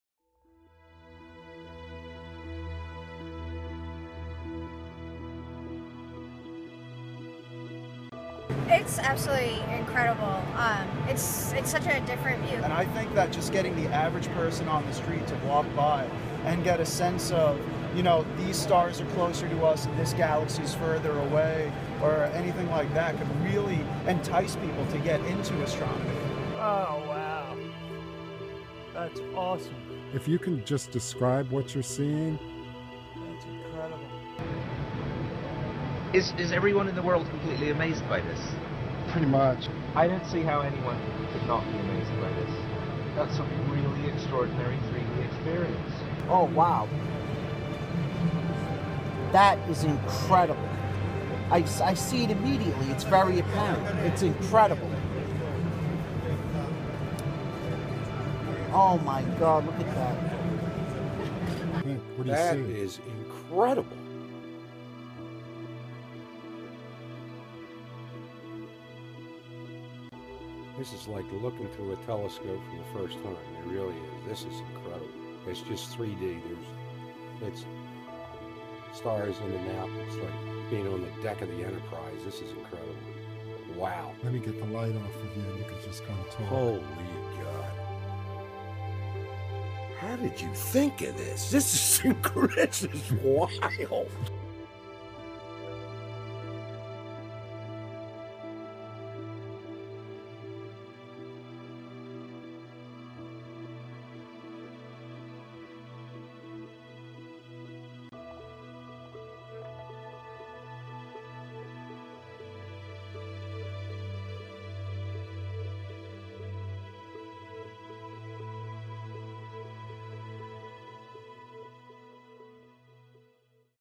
Gift for Men! Space Walker 3D Binoculars
New Patent Pending! The Wolrd's First and Only 3D Astro Binoculars. See The Universe in Glorious 3D. Easy to Use, no Astronomy Knowledge Required. Fanbtastic Gift For The Upcoming Holidays!